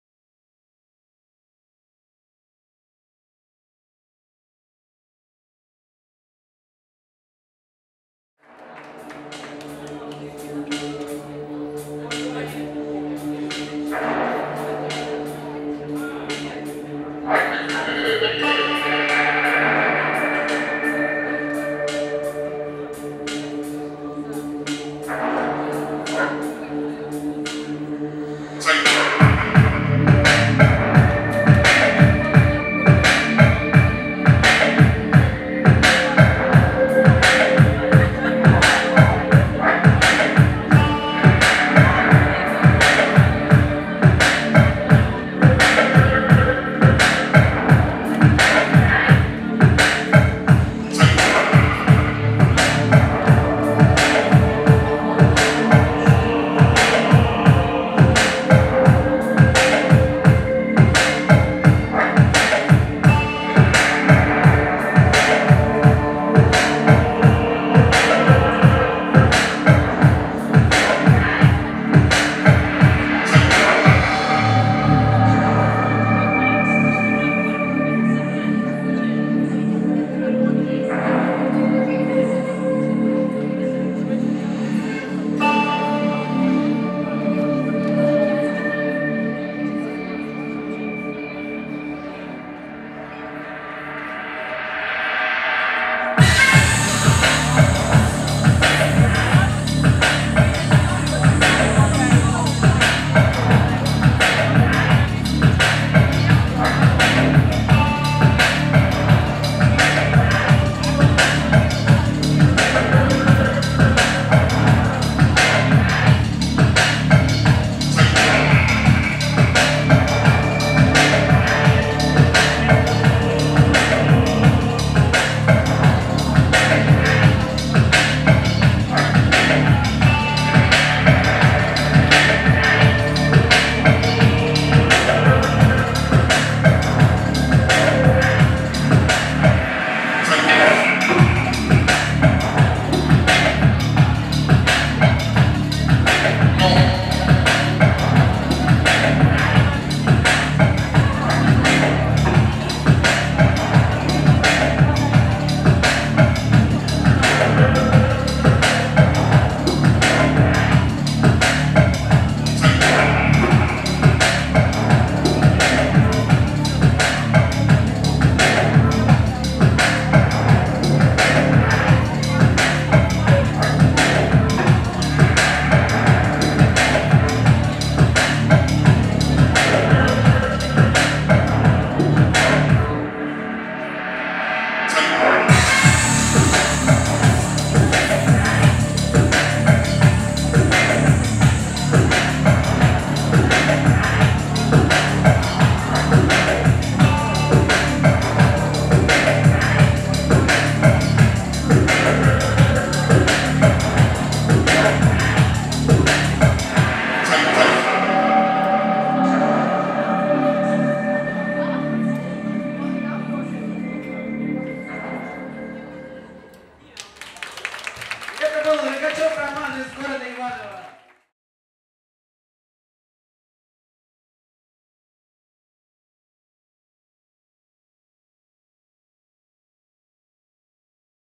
Korolevskiy DB V, jun 2015 - Rogachev Roman
Рогачев Роман, г. Иваново (Exotic pole dance, solo, профессионалы)

Элитный танцевальный конкурс я по клубным направлениям - GO-GO, STRIPDANCE и EXOTIC POLE DANCE.

г. Москва. клуб "Сапфир"
14 июня 2015 года